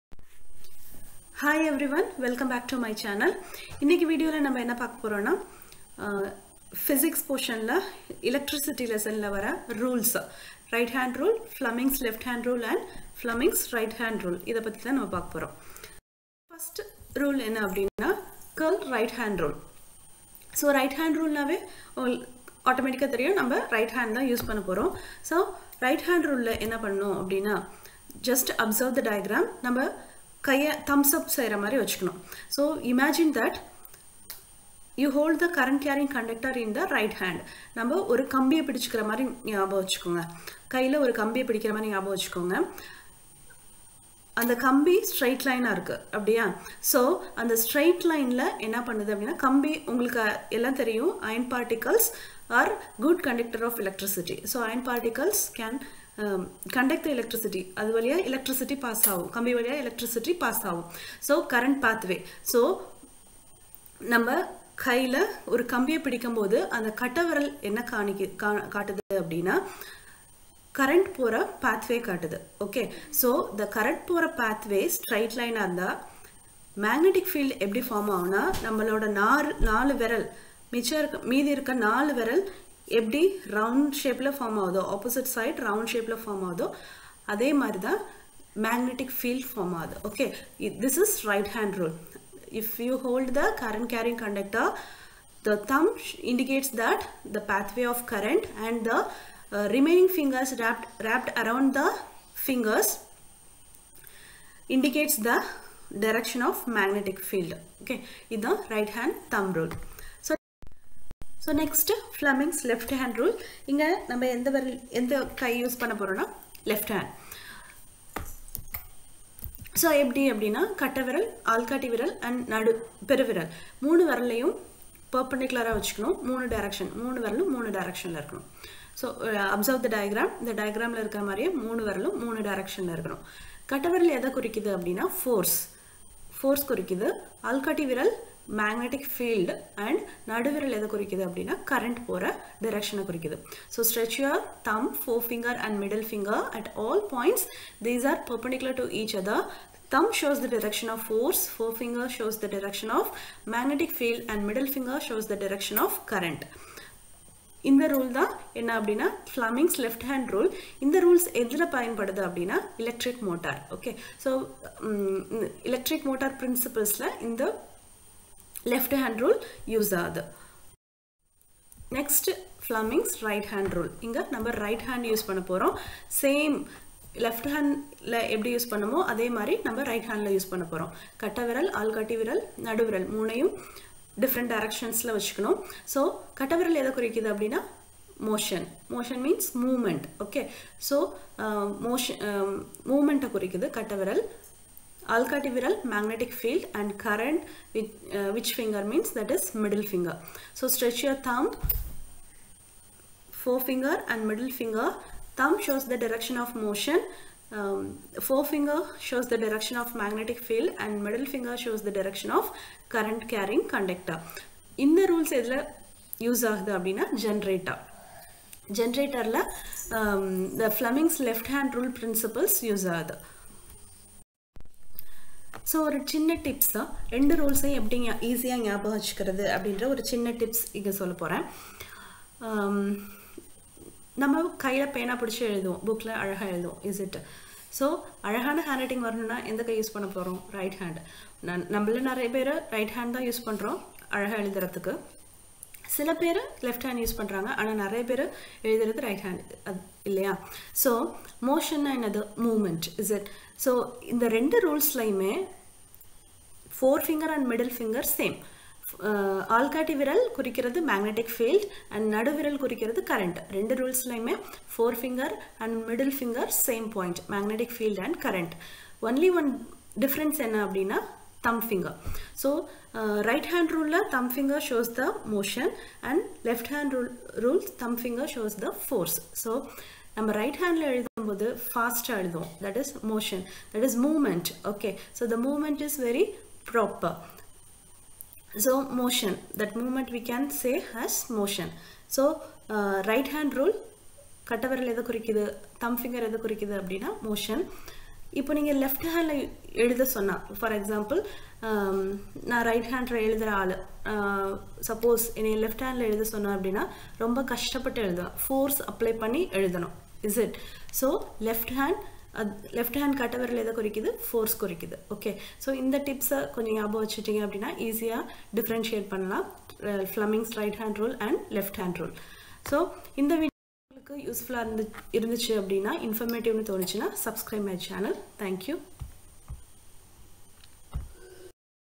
Flemmingsrules || curl right hand rule || tipsandtricks தமிழில்...@learneasy1707
In this video explain about curl right hand rule Fleming's left hand rule and Fleming's right hand rule and also given tips to remember Fleming's left hand rule and right hand rule.